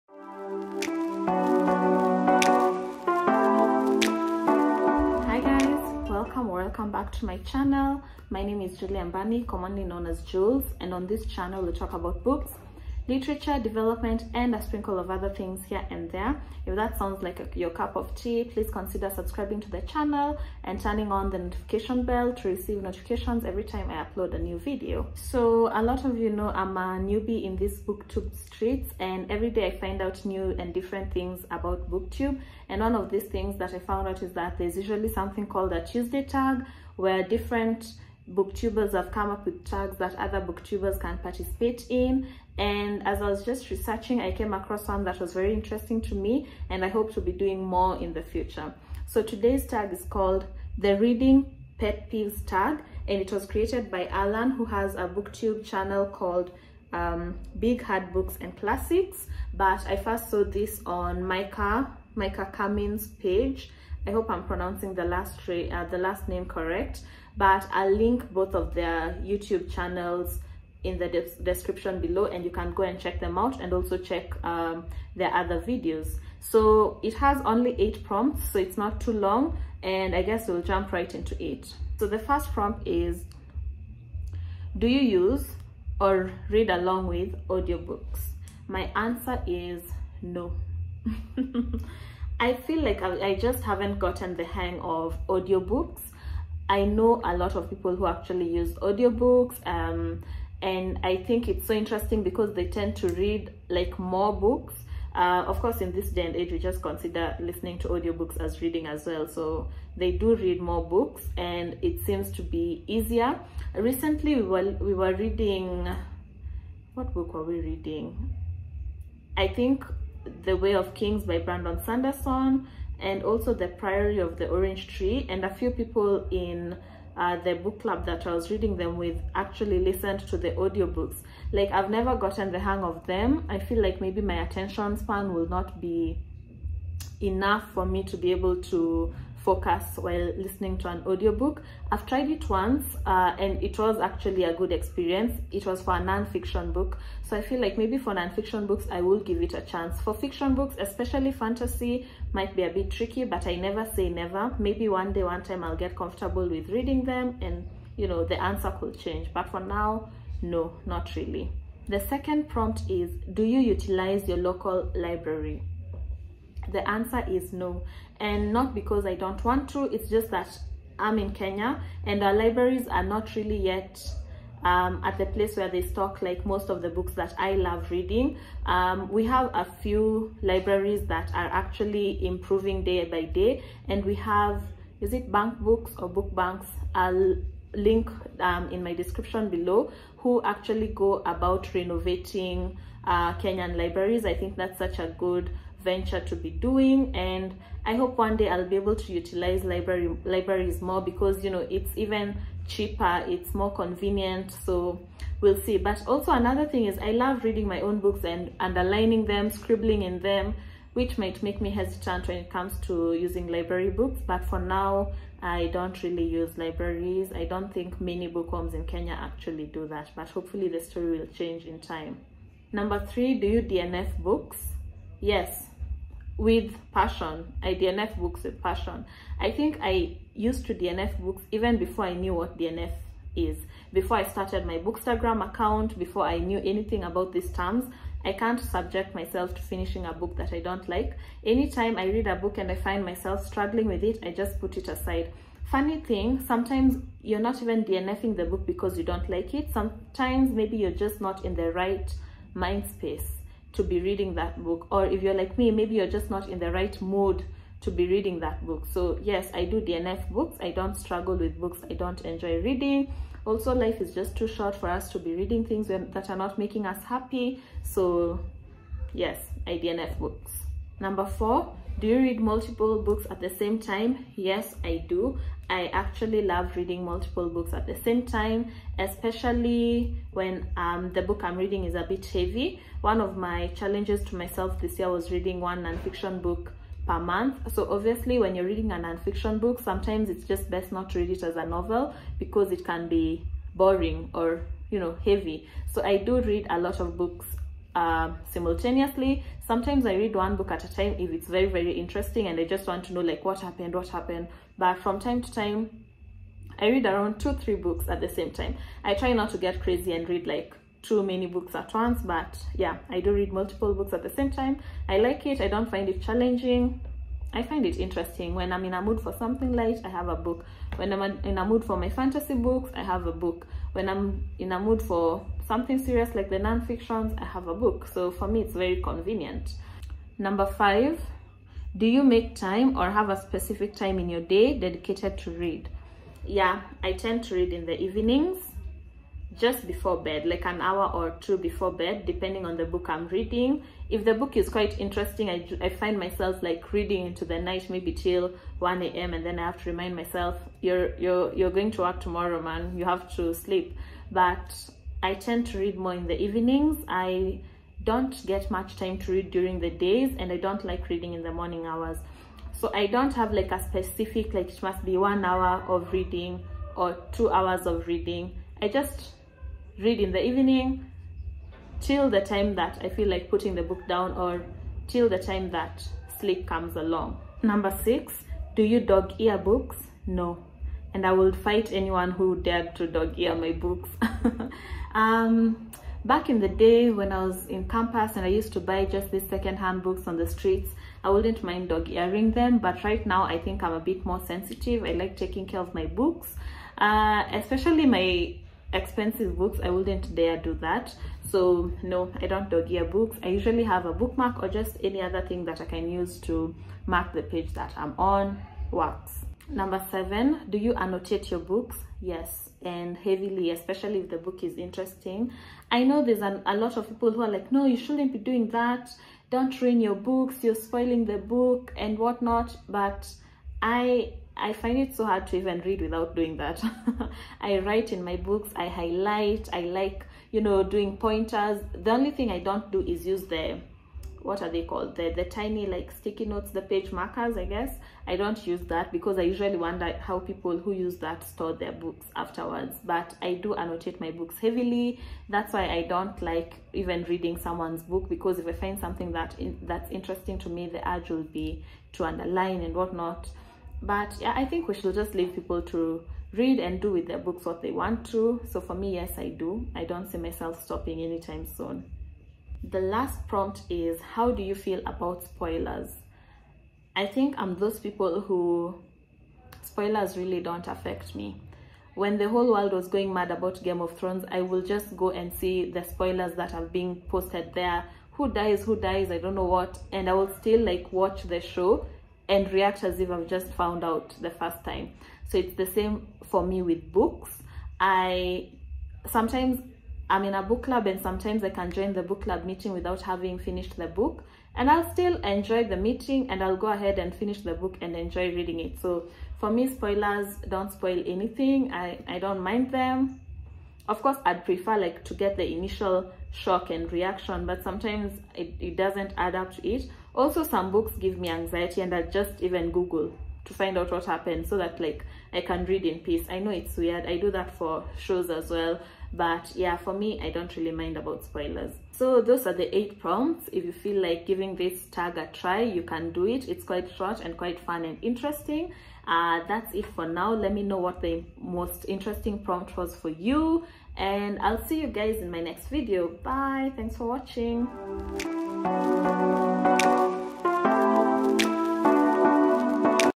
The Reading Pet Peeves Tag! || Tag Tuesday
Hi friends!

It’s Tag Tuesday and today we’re doing The Reading Pet Peeves Tag!

Prompts:

1. Do you use (or read along with) audiobooks?
2. Do you utilize your local library?
3. Do you DNF books?
4. Do you read multiple books at the same time?
5. Do you make time, or have a specific time, in the day dedicated to read?
6. Do you dog-ear books?
7. Do you annotate your books?
8. How do you feel about spoilers?

I nominate anyone who would wish to participate!

For those in Kenya, you can buy any books from Nuria Bookshop’s and if you use my code: NURIAXJULES you get a 7% discount on each and every book that you buy

Cheers and remember, be kind 🎊